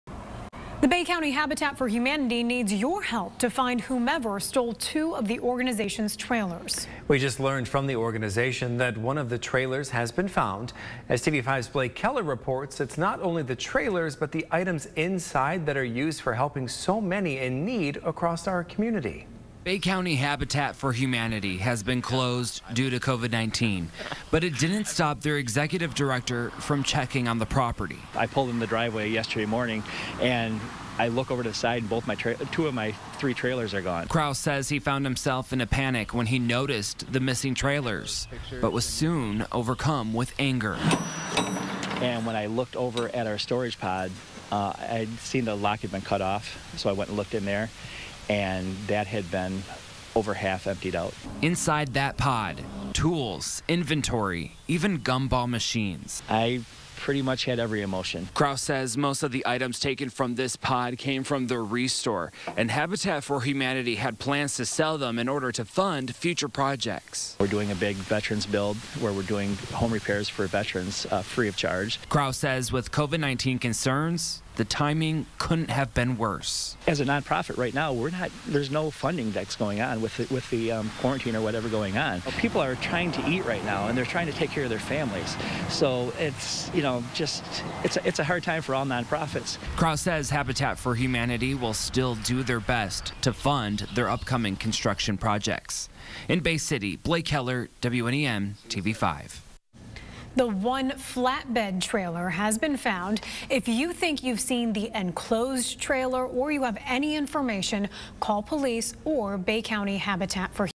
WNEMTV5 - Habitat for Humanity searching for stolen trailers, items
TV5’s Blake Keller speaks with the Bay County Habitat for Humanity, who says trailers and items in their storage pods were stolen overnight.